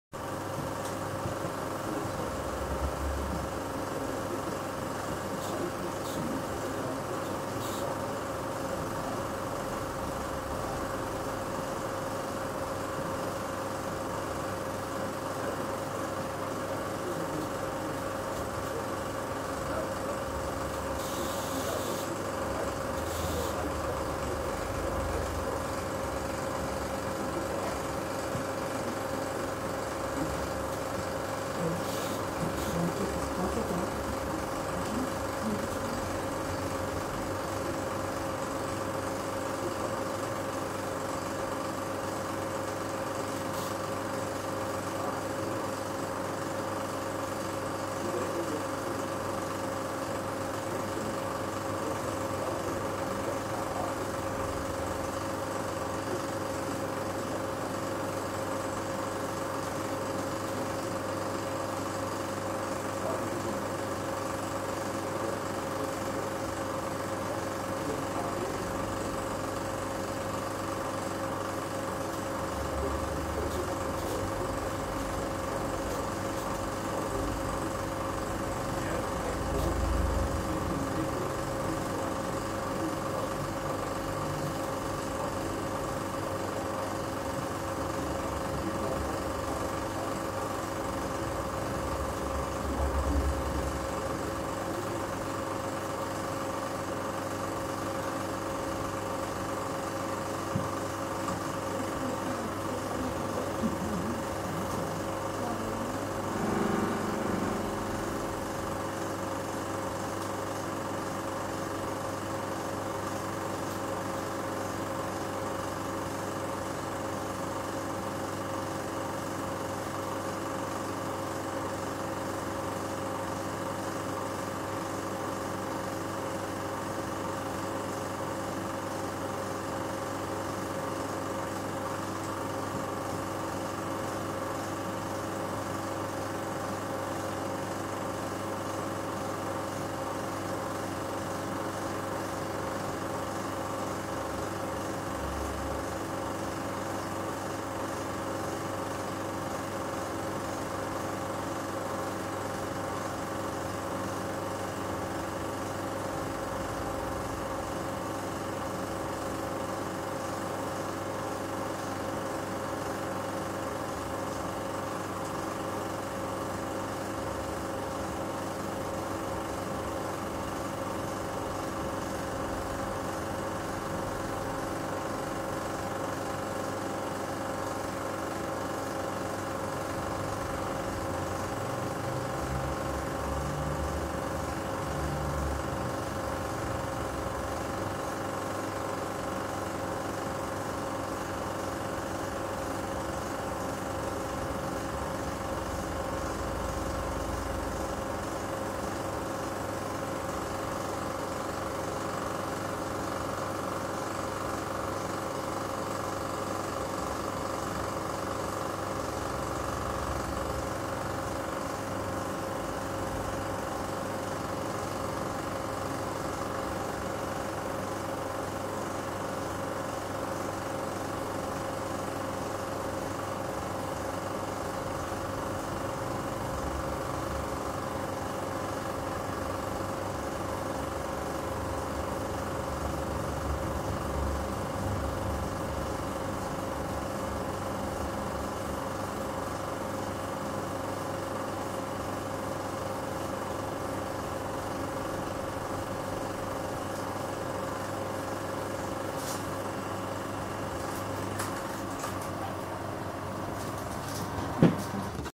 Very early american hovercraft (GEM) experiments with a 6' model. Ca. 1962 footage.
Very early GEM (ground effect machine - hovercraft) patio test using a 6' model from an american hovercraft pioneer company, from Huntington Beach,CA. Footage taken from a old 16 mill. reel @ Museum Marxzell - 100% worth a visit - ! No sound but the one from the projector ... sorry for the bad picture quality, but this is a pretty old movie, first time taken out of its cover since the seventies ;) ! ENJOY !